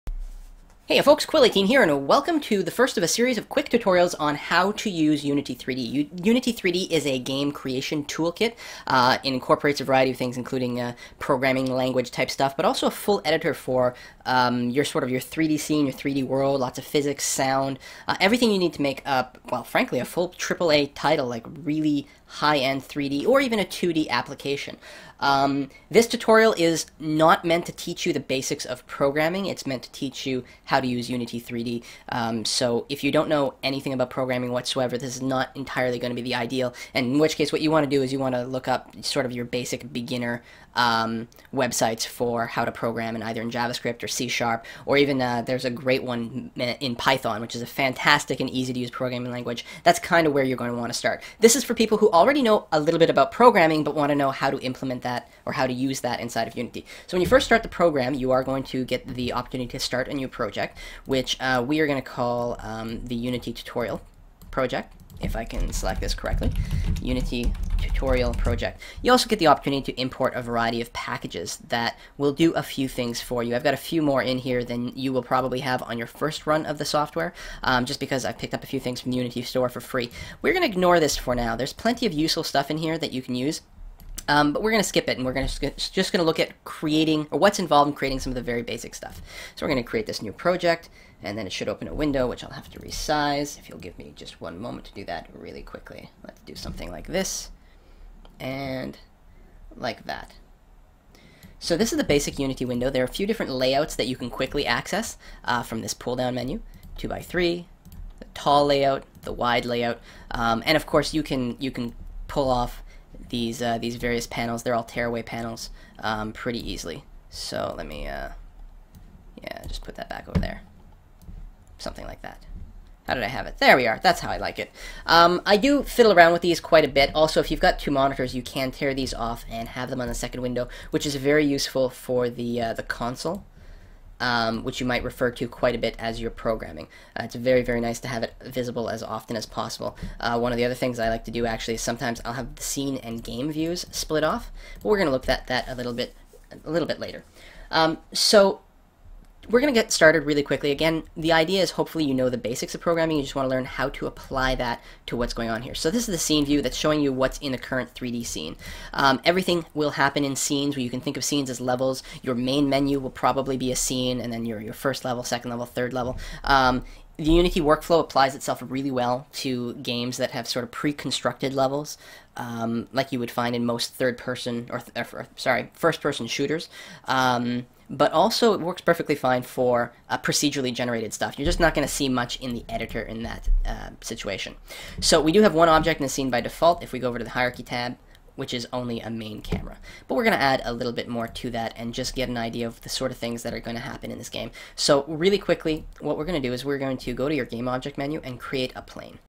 Unity 3d Tutorial for Programmers - Part 1 - New Project Setup
Step by step directions on how to make a 2d-looking game using the 3d Unity Game engine, including how to use the physics engine.

COMPLETELY NEW TO PROGRAMMING?  I'd suggest you start with this great free guide to making games in Python.  Very beginner friendly